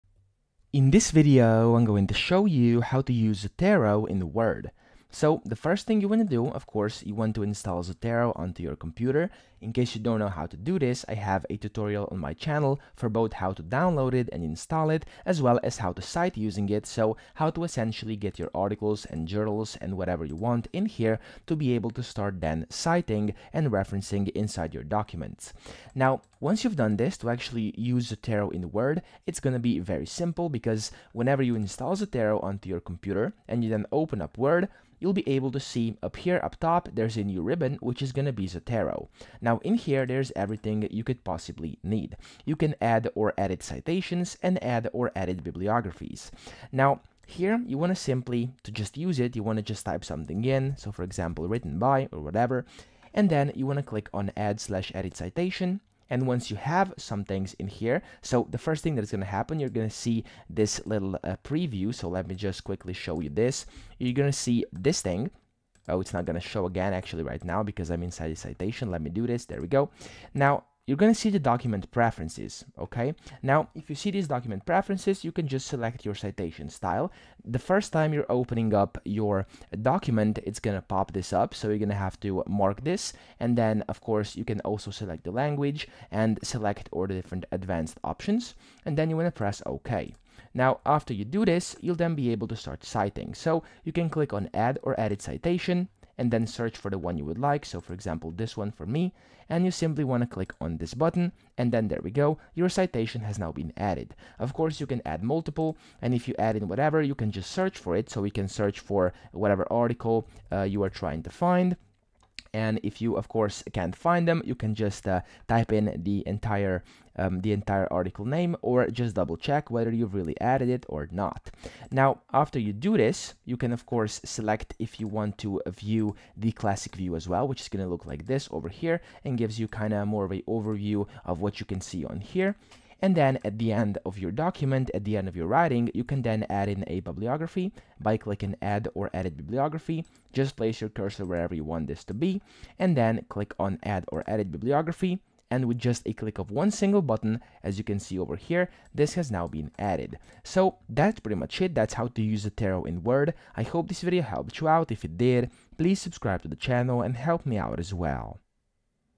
How To USE Zotero In Word (QUICK & EASY) 2026
In this video, I’ll show you how to use Zotero in Word quickly and easily. This is a full guide on how to insert citations and build your bibliography using Zotero inside Microsoft Word in 2026.

If you’ve been trying to integrate your Zotero library into Word for seamless referencing, this is the video you need to see!